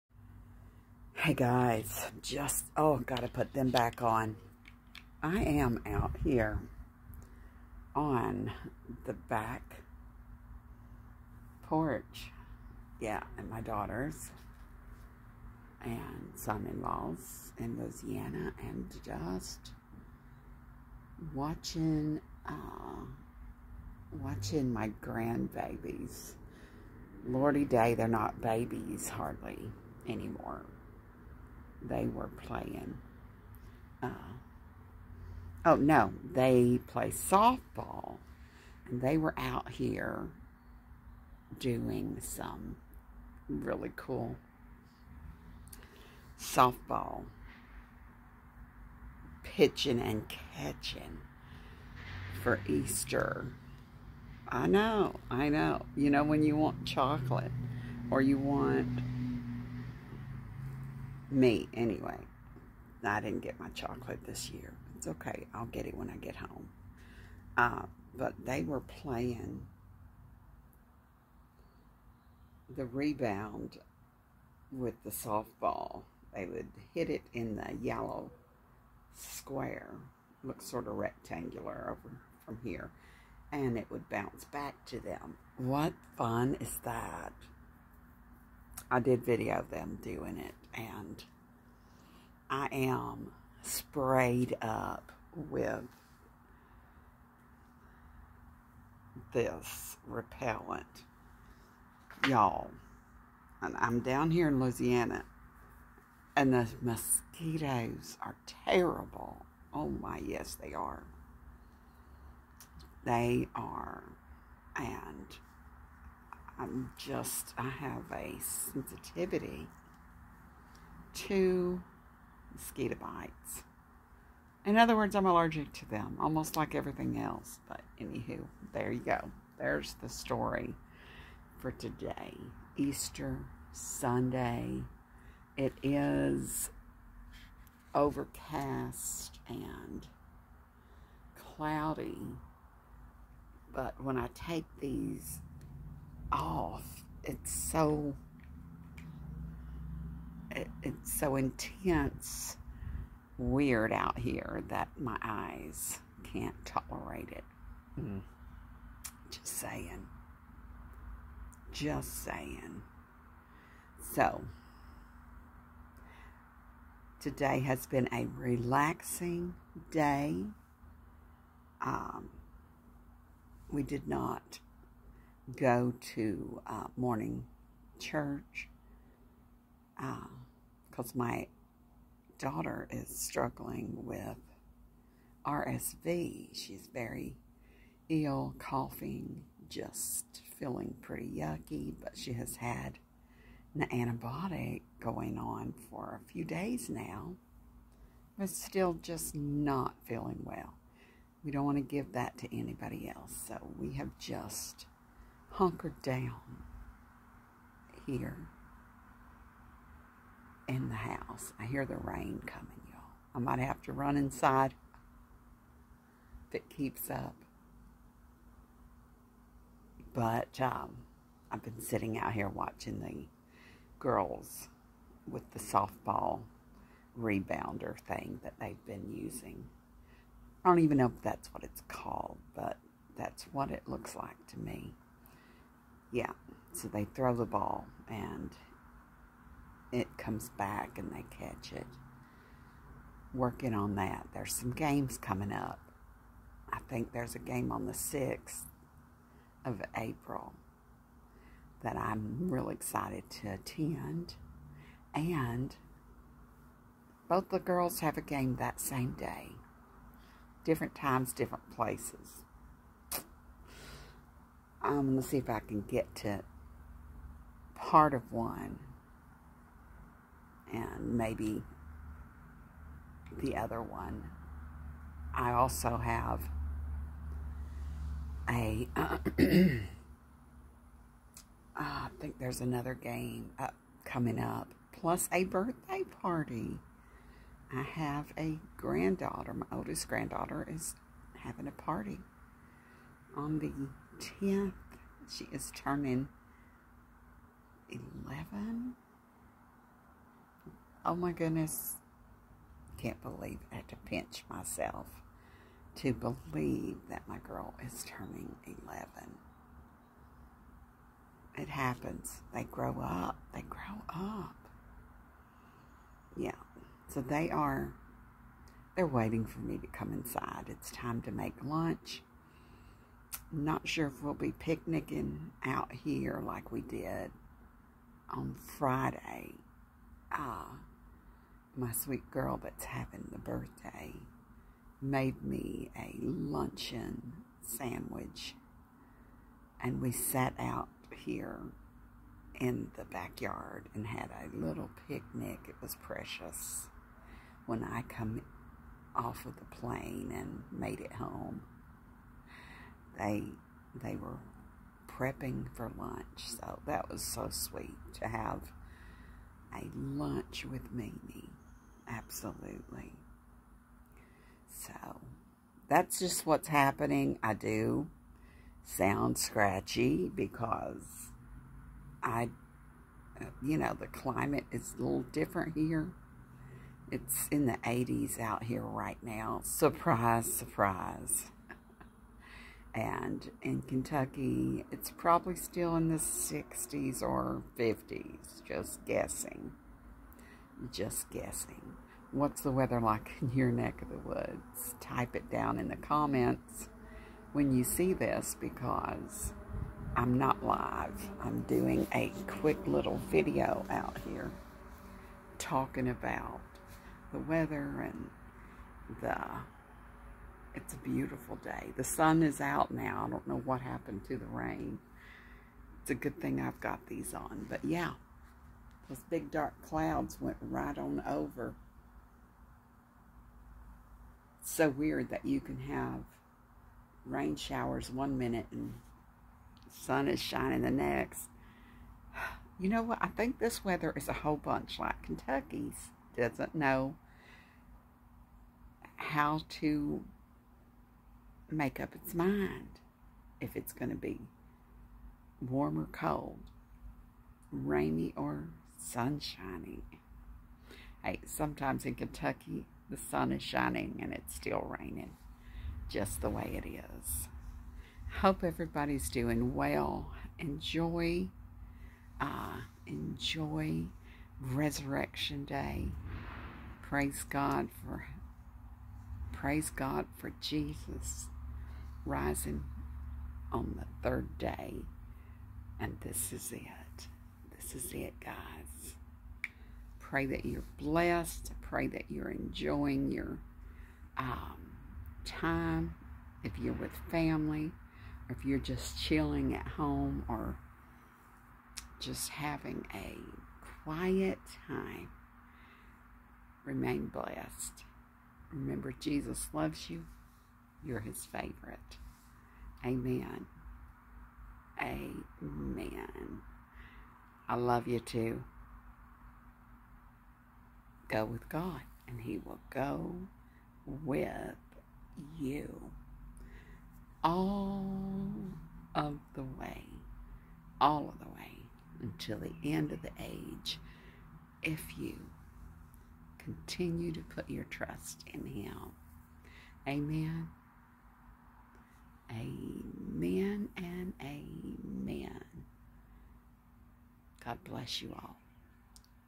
Greetings This Easter Sunday!
Happy Resurrection Day everyone...God is so good. Keep prayers coming for my daughter recovering from RSV. She is really struggling with it. Much love and blessings to all!